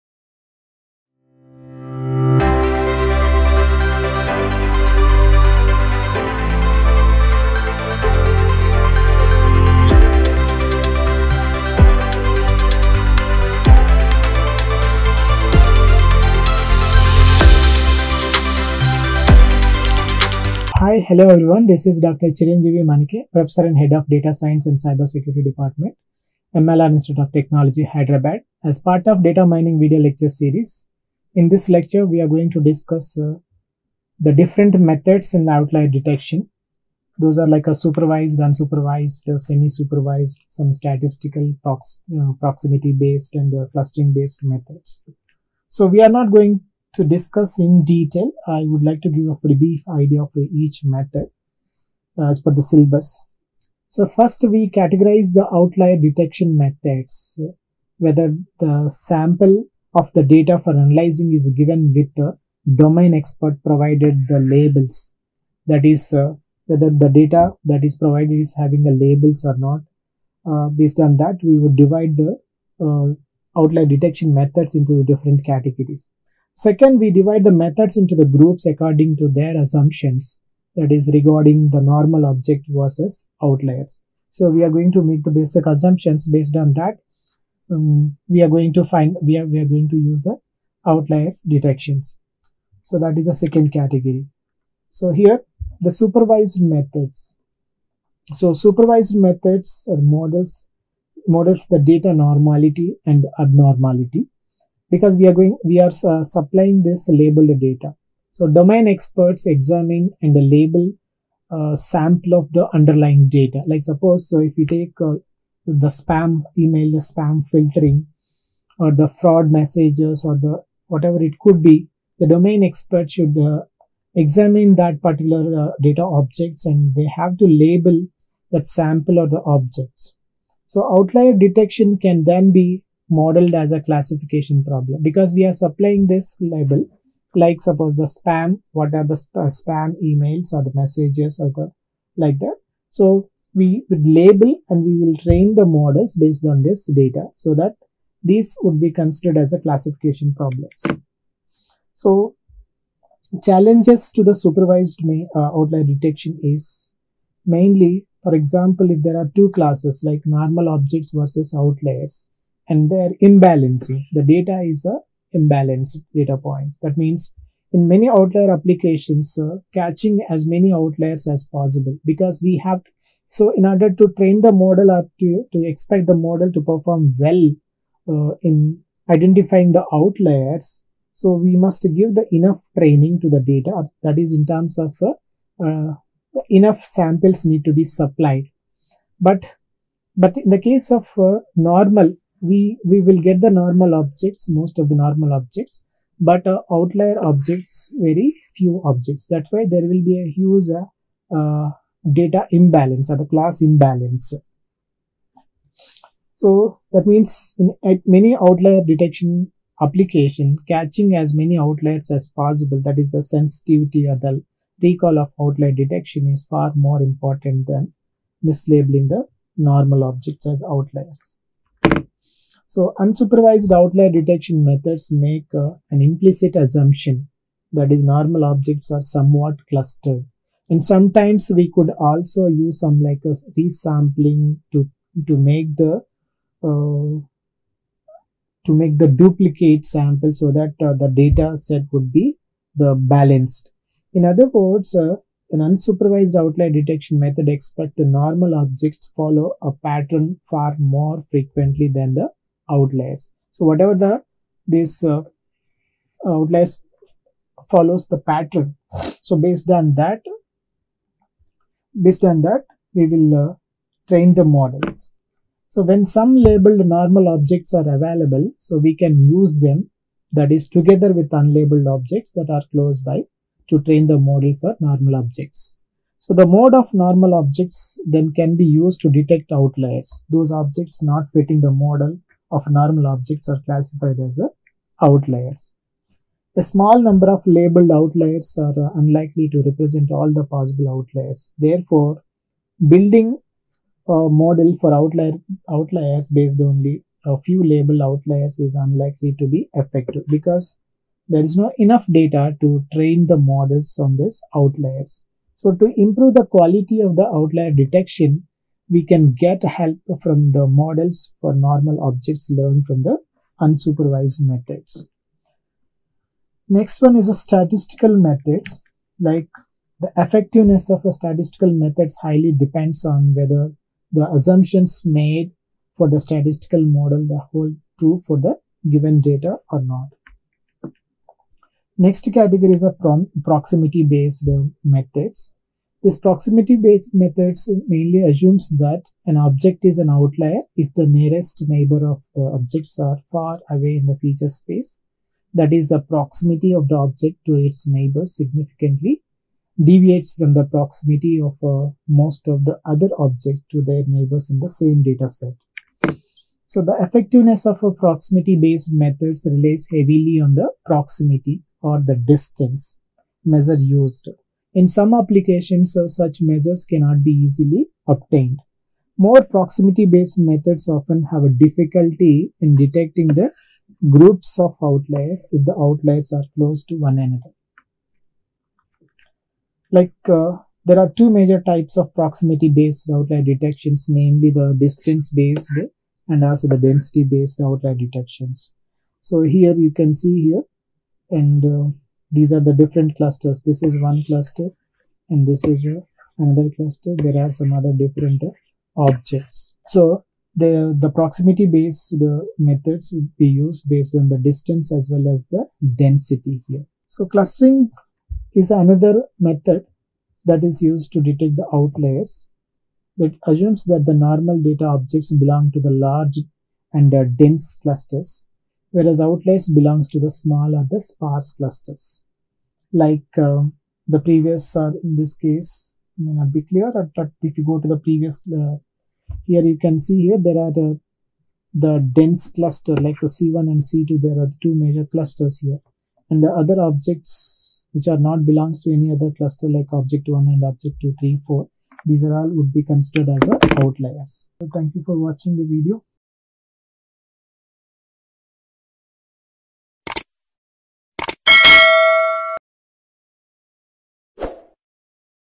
LEC45| Data Mining | Outliers and Outlier Analysis Different Methods by Dr. Chiranjeevi Manike
Professor & Head,
Department of CS & DS,
 MLR Institute of Technology , Hyderabad.